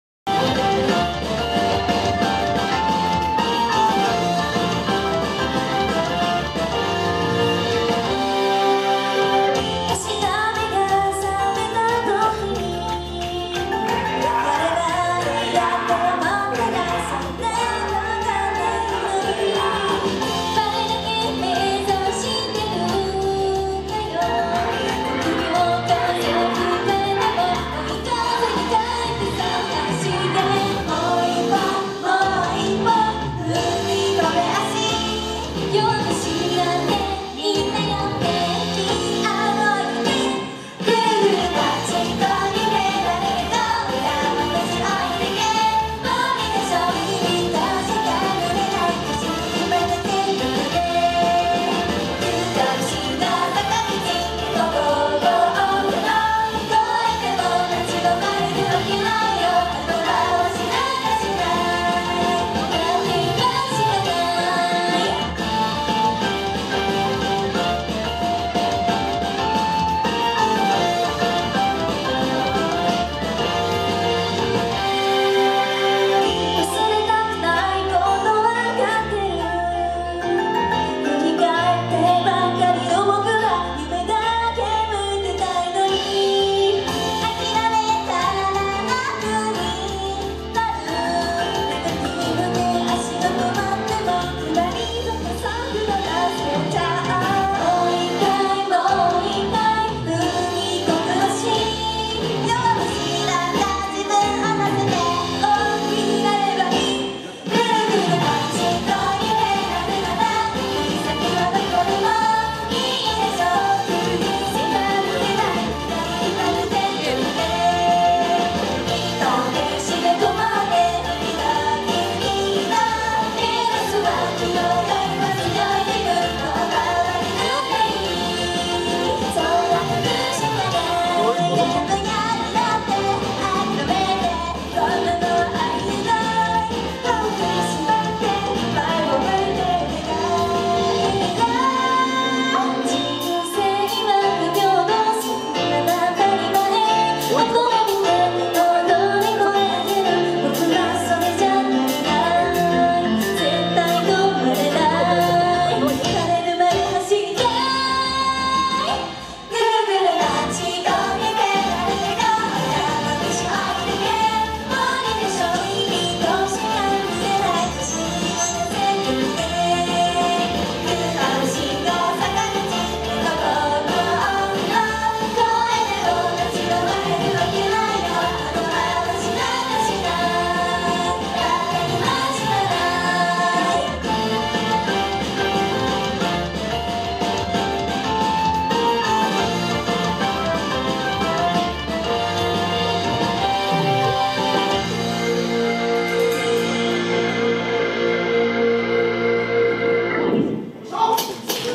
【ライブ映像】こみっきゅおん !.@群馬県庁群馬会館室内特設ステージ(こみっきゅおんさんのライブステージ!!)
第一回にして全国より総勢32組のアイドルユニットさんが・・・この超が付く程のスーパーローカルシティ前橋に集結致しました。自分は群馬会館室内特設ステージのセンターに陣取りました!!。